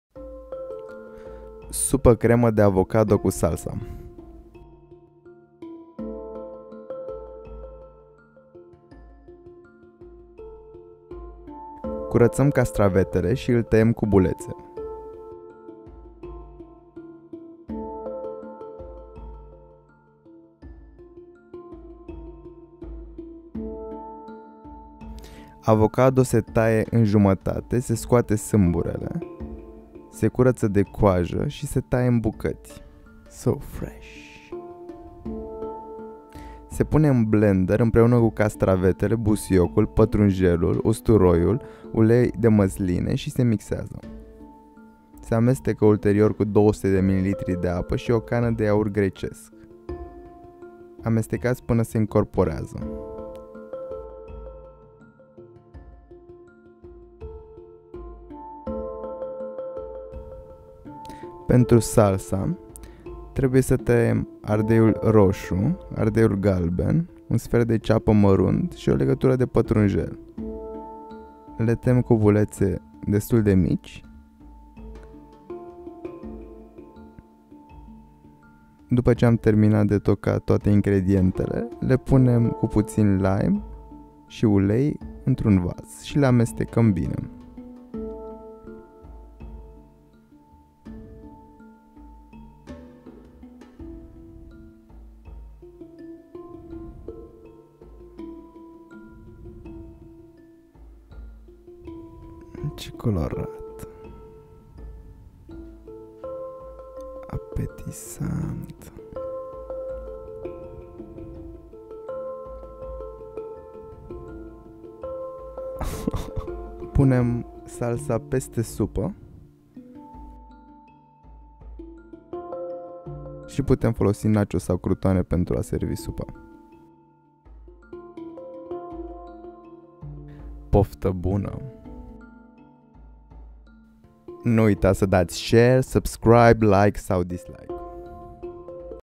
Spice speed and FEED - Supa crema de avocado cu salsa |RO
Sites

Iulian Popa

Autori

Madalina Popa - retete si preparare
Iulian Popa - filmare, editare, voce
Reteta

Supa:
- 1 avocado copt
- 1 castravete
- suc de la jumatate de lime
- 2 linguri busuioc si patrunjel
- un sfert de cana de iaurt grecesc
- 1 catel de usturoi
- 1 lingura de ulei de masline
- 200 ml apa
- sare
- piper

Salsa:

- un sfert ardei rosu
- un sfert ardei galben
- un sfert de ceapa
- o lingura patrunjel tocat
- o lingura ulei de masline
- 1 lingurita zeama de lime
- sare
- piper